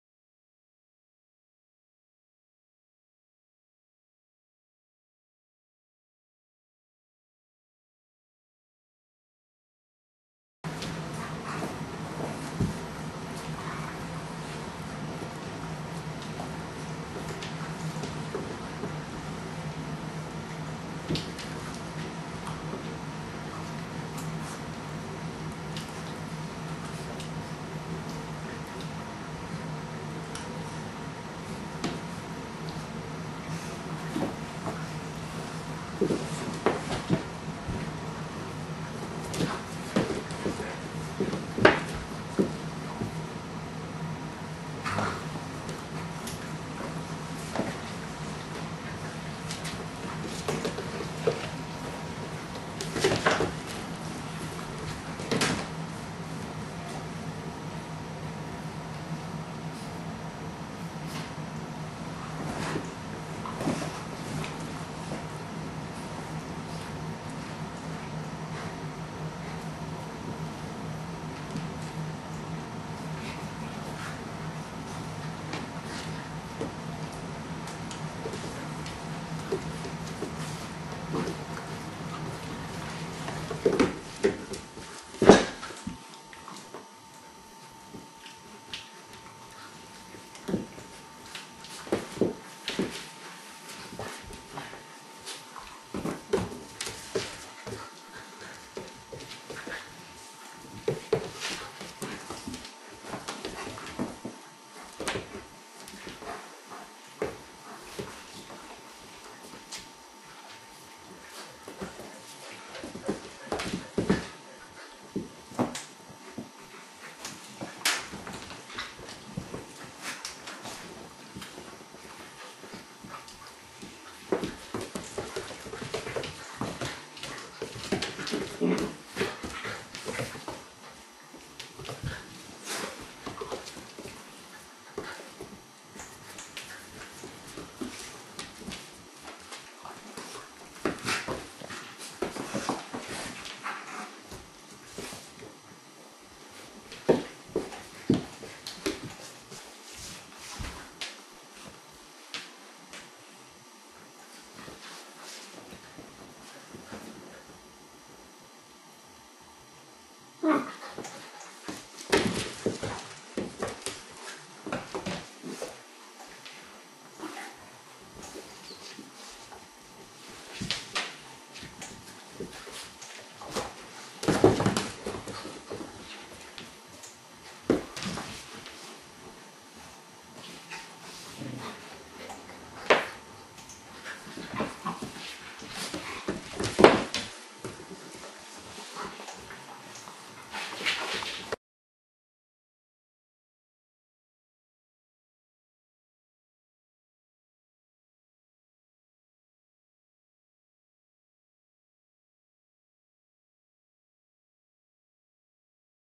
duke, Willy, & Ana 2
Great Pyrenees males playing with Kuvasz puppy